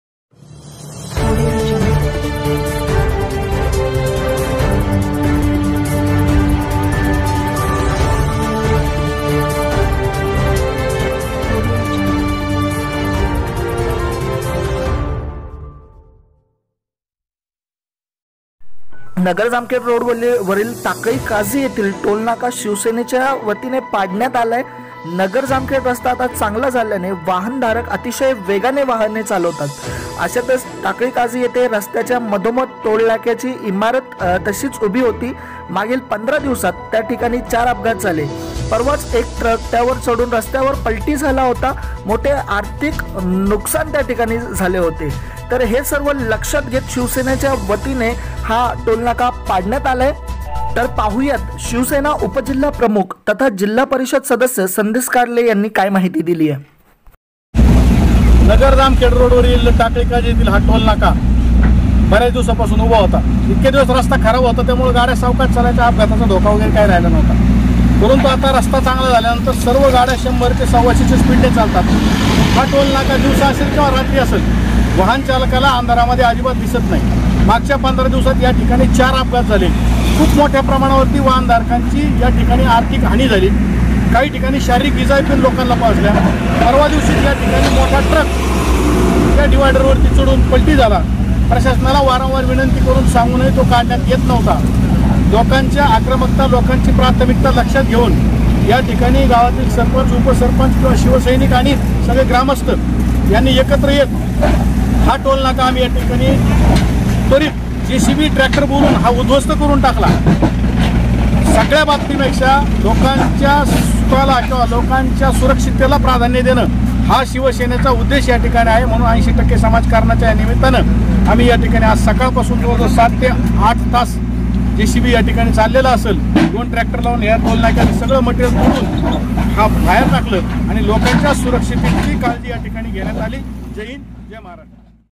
नगर जामखेड रोडवरील टाकळी काझी येथील टोल नाका शिवसेनेने पाडला
नगर जामखेड रस्ता आता चांगला झाल्याने वाहन धारक अतिशय वेगाने वाहने चालवतात अशातच टाकळी काझी येथे रस्त्याच्या मधोमध टोल नाक्याची इमारत तशीच उभी होती मागील पंधरा दिवसात त्या ठिकाणी 4 अपघात झाले परवाच एक ट्रक त्यावर चढून रस्त्यावर पलटी झाला मोठे आर्थिक नुकसान झाले,सदैवाने प्राणहानी झाली नाही सदरची माहिती टाकळीचे ग्रामस्थ यांनी शिवसेना उपजिल्हा प्रमुख तथा जिल्हा परिषद सदस्य संदेश कार्ले यांना सांगितली त्यांनी तात्काळ त्या ठिकाणी जाऊन सदर बाब महामार्ग अधिकाऱ्यांना सांगितली त्यांनी काऱ्यवाही करतो असे सरकारी उत्तर दिले अजून अपघात होऊन प्राणहानी होण्यापेक्षा स्वतः संदेश कार्ले व शिवसैनिकनिकांनी आक्रमक होत ताबडतोब जे .सी.बी व ट्रॅक्टर बोलावून टोल नाका उद्धवस्त केला व जनतेचे अपघात होण्याचा धोका टाळला.
     शिवसेनेच्या या धडक कृतीमुळे नागरिकांमधून समाधान व्यक्त होत आहे यावेळी शिवसेना उप तालुकाप्रमुख शंकर ढगे, टाकळीचे उपसरपंच अविनाश पवार, अशोक ढगे, संभाजी आटोळे, विशाल ढगे, सदाशिव म्हस्के,हेमंत गावडे,हर्षल ढगे, संतोष ढगे व ग्रामस्थ यांच्या उपस्थितीत टोल नाका पाडण्यात आला.
*नारायणडोहो प्रतिनिधी रफिक शेख*